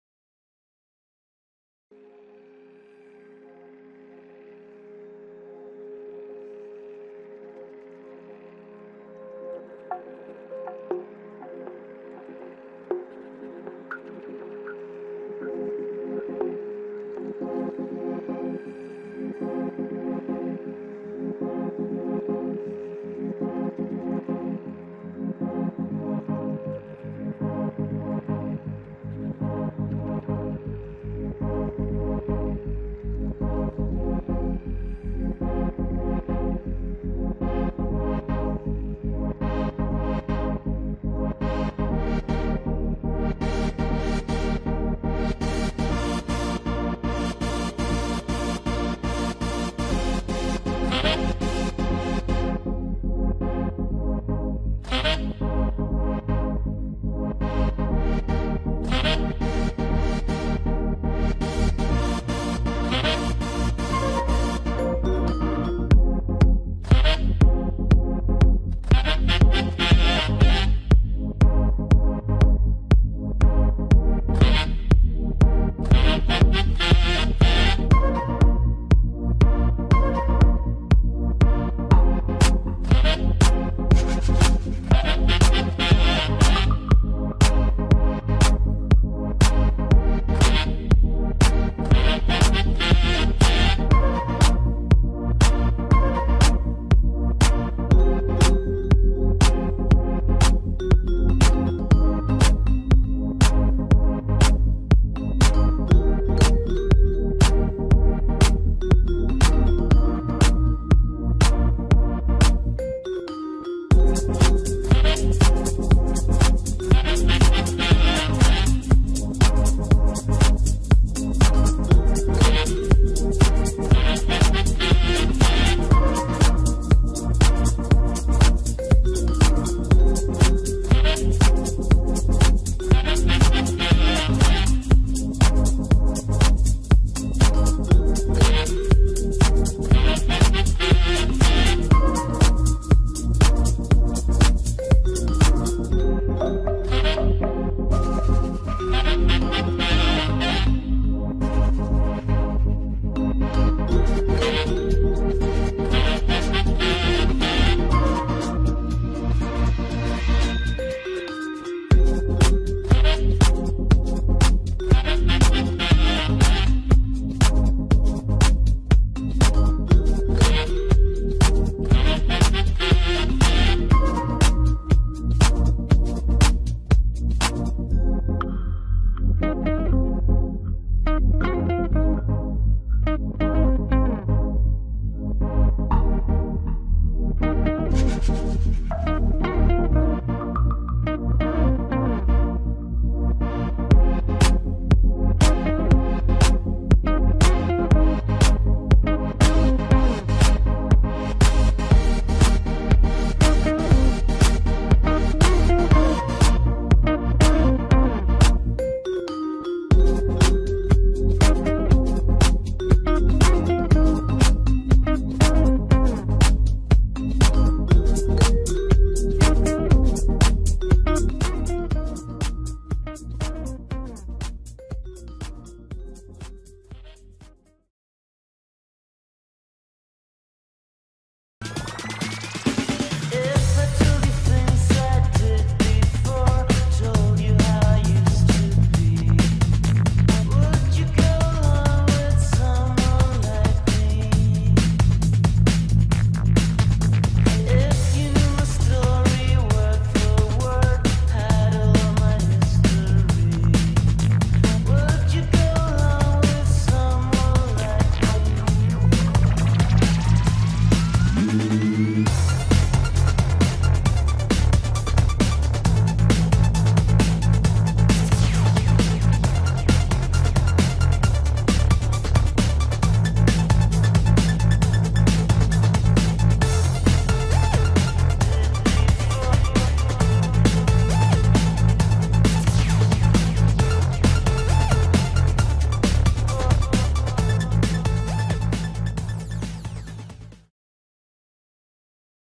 Thermals
a great day to fly in thermals. it was a hot summerday in august 2012.